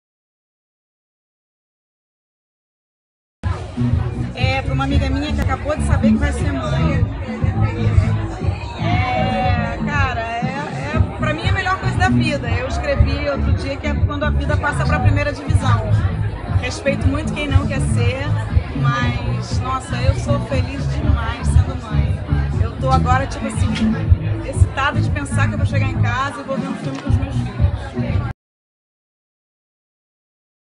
De mãe para mãe - Maria Ribeiro
Perguntamos às famosas o que elas diriam para uma amiga que acabou de descobrir que esta grávida. Veja a resposta!